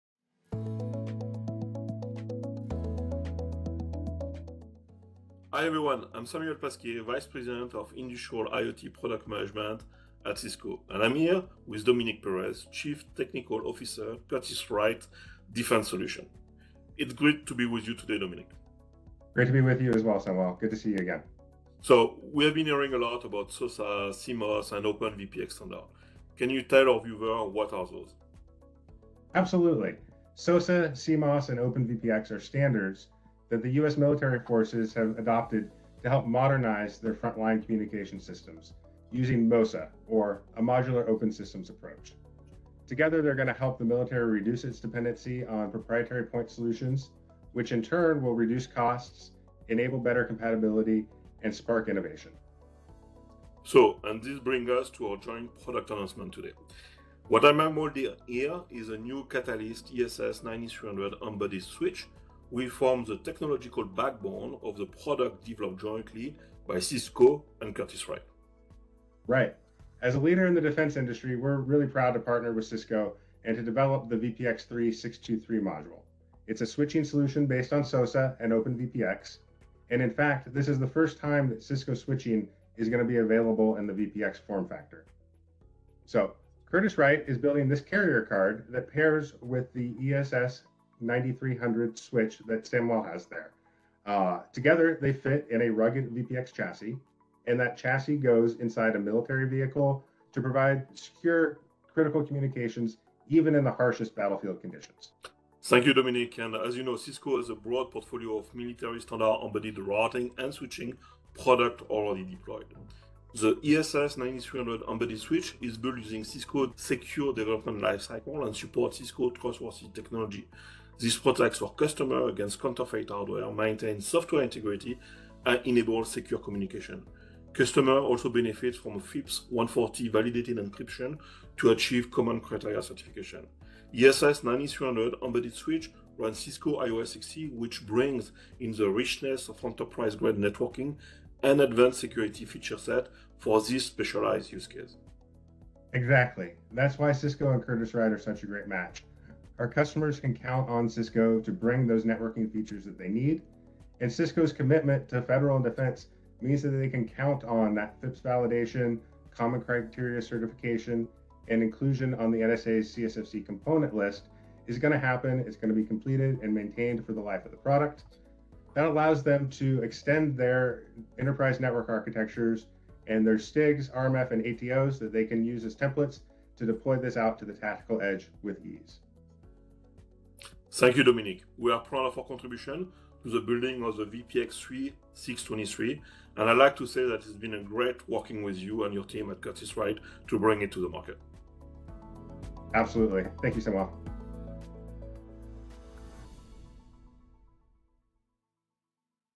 Cisco and Curtiss-Wright unveil CMOSS/SOSA and OpenVPX compliant switching solution
The new VPX3-623 switch, based on the Catalyst ESS9300 Embedded Series, adopts CMOSS, SOSA, and OpenVPX standards—an open computing and networking architecture—ushering in a new era for fulfilling demanding military computing and communication needs.